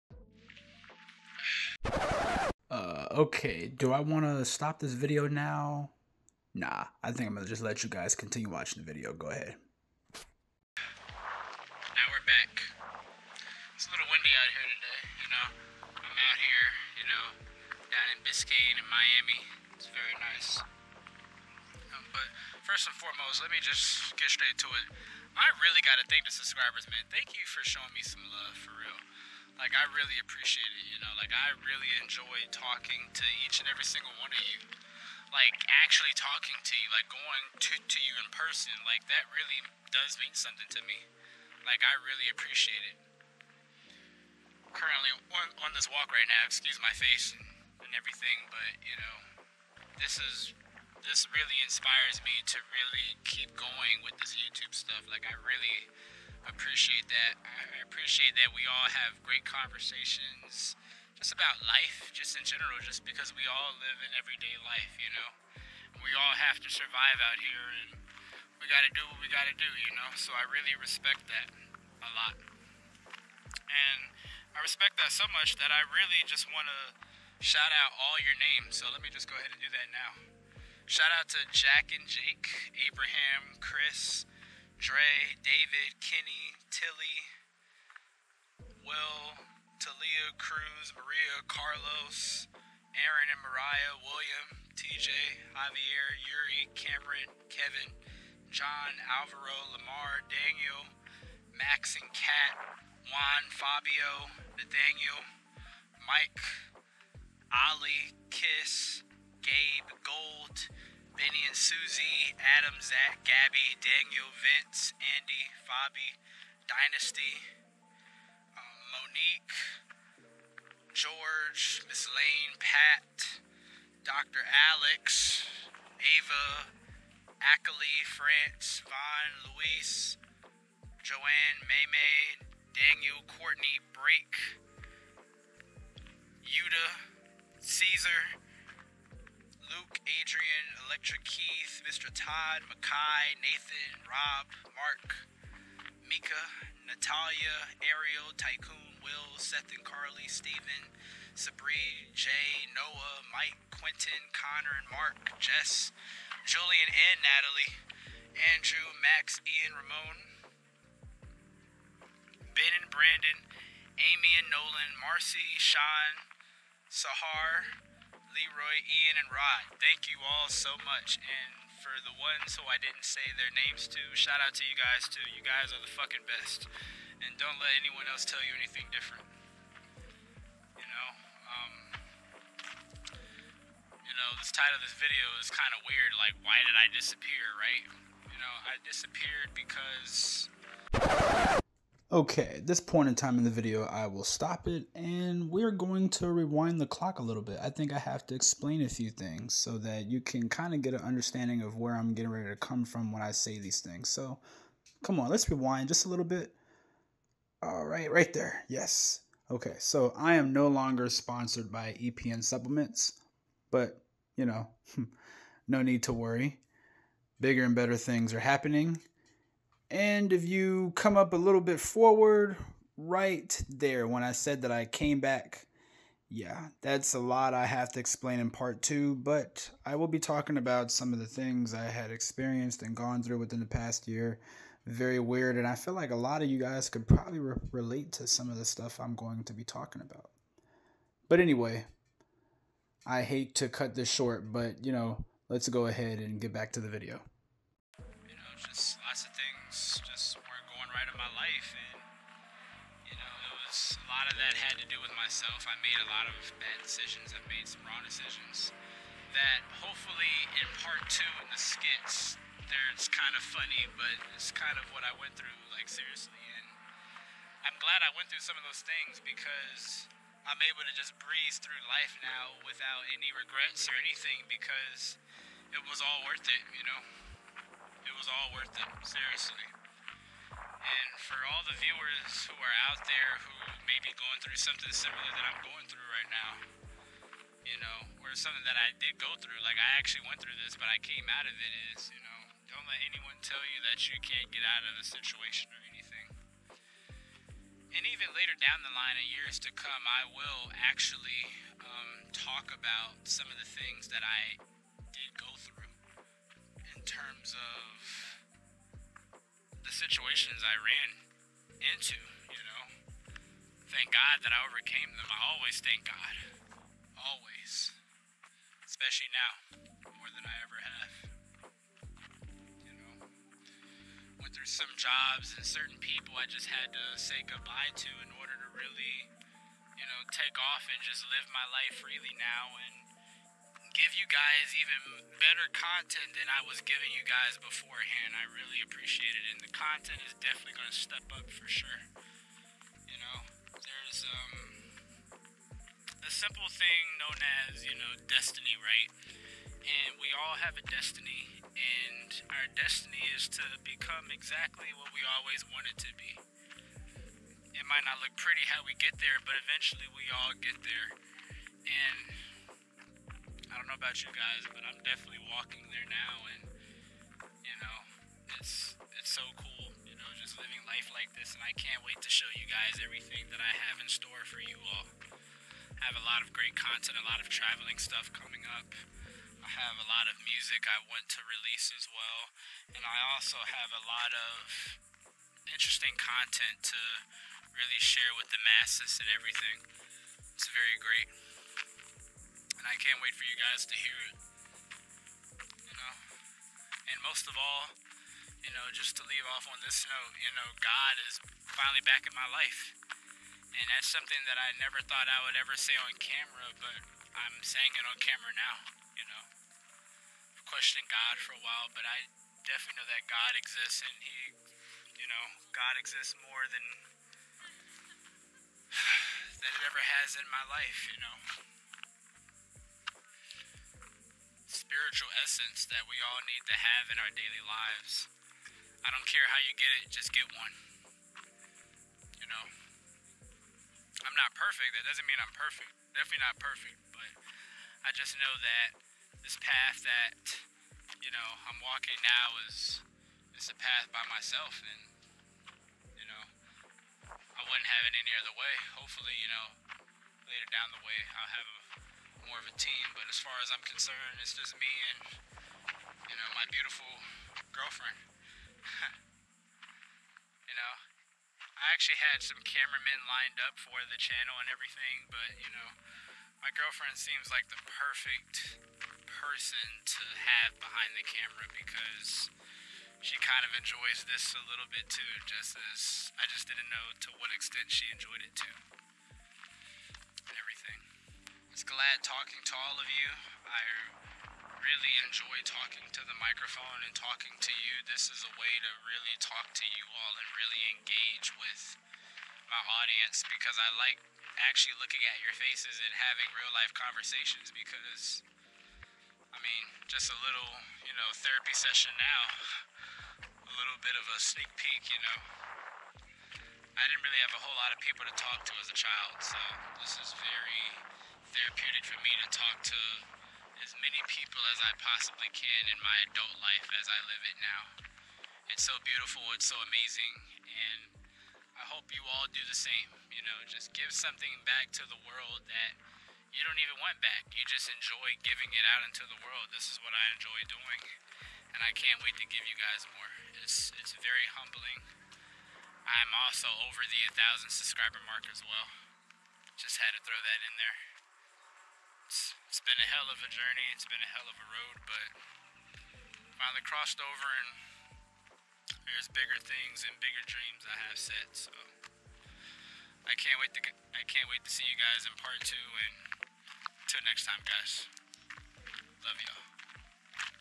Why I Disappeared Part 1
Thank you all for helping me reach the 1k Mark. It's been a year coming and now I'm back in YouTube full swing. Let's reach that next goal which is 5k. Sorry for leaving ya'll but lots of content coming your way.